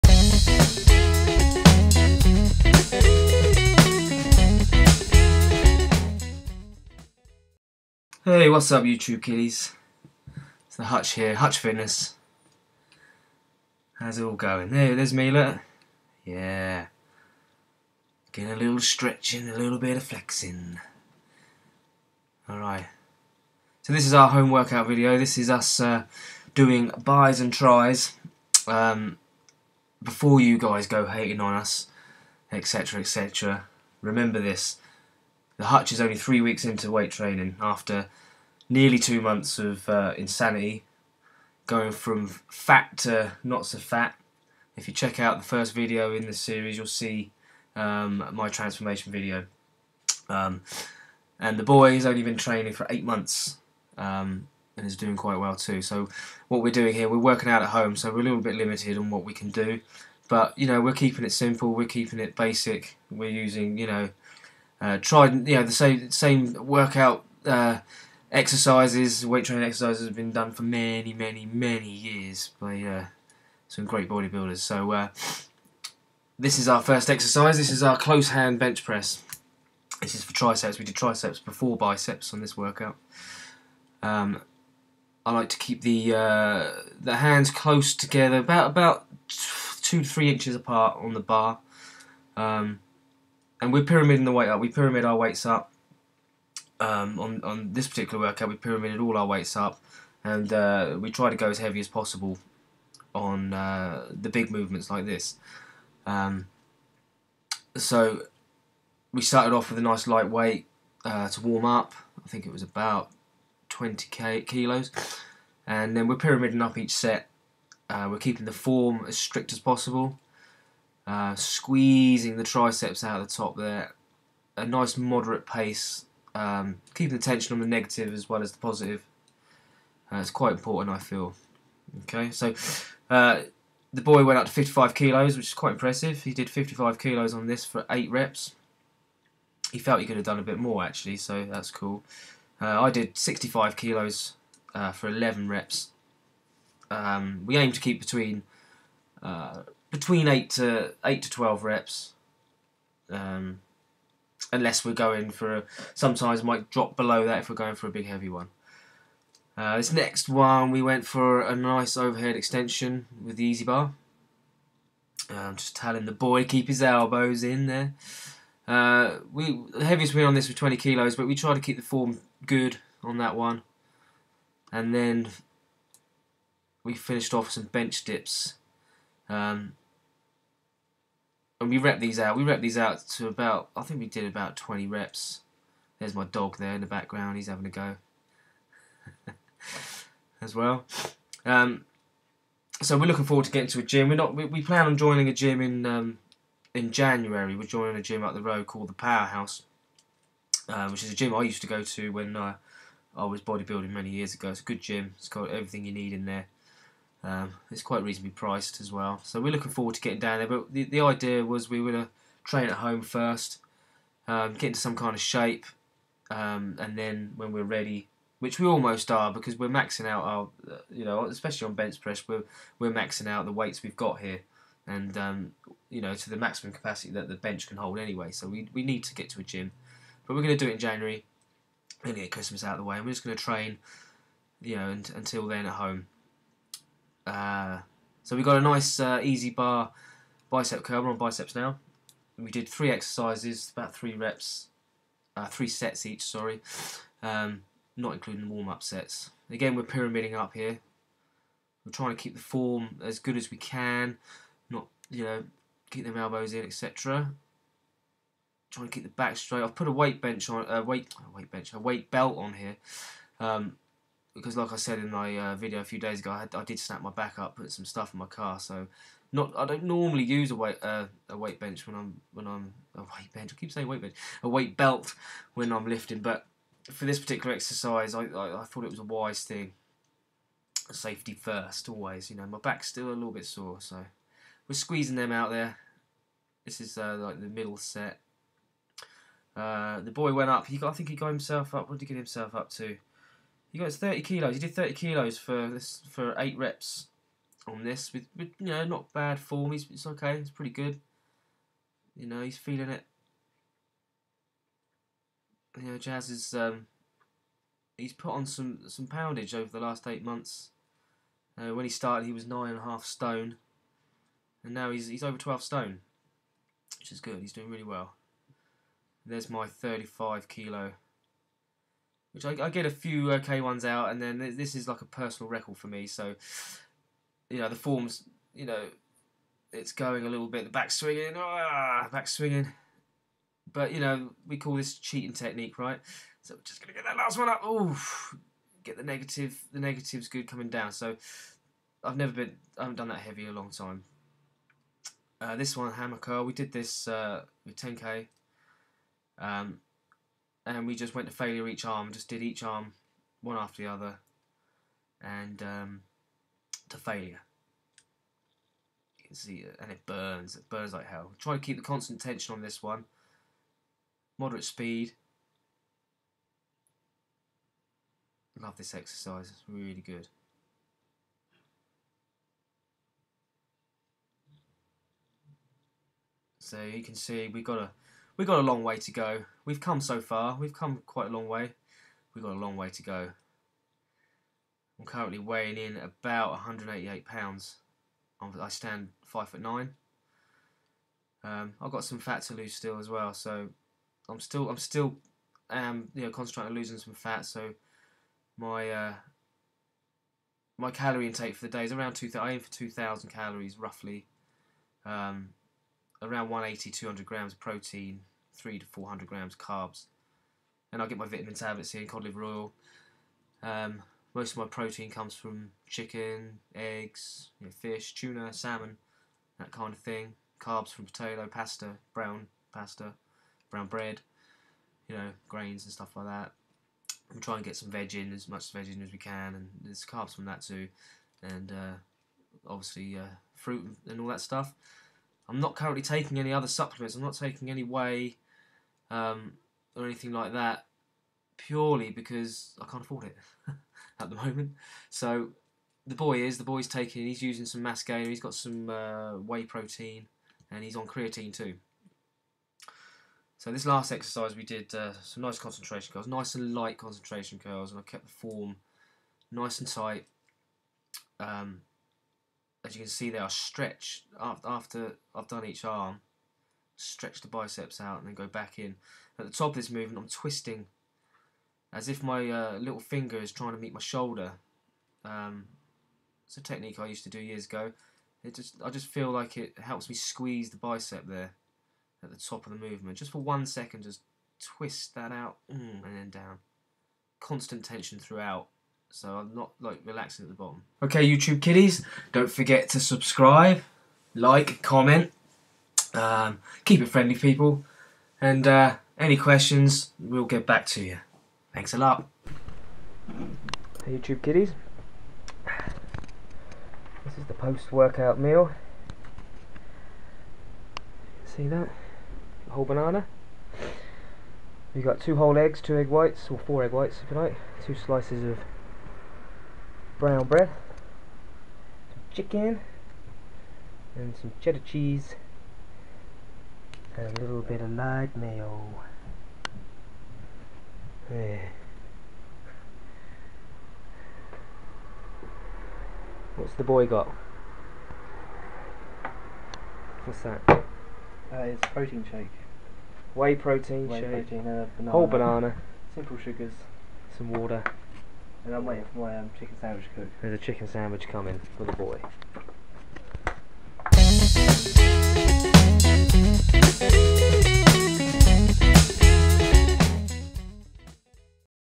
Biceps and Triceps Bodybuilding at Home
Hey youtube kiddies!!

This is one of our bicep and tricep workouts.

We have a long way to go!! but we are dedicated to reaching our goals. If you are new to bodybuilding and fitness you may get some useful info from us? or not !

Any question you have we will answer, and any advice you may have for us we are happy to hear, as long as it's constructive :)

Instead of Thumbs down, just move on out.  Na seriously we don't mind !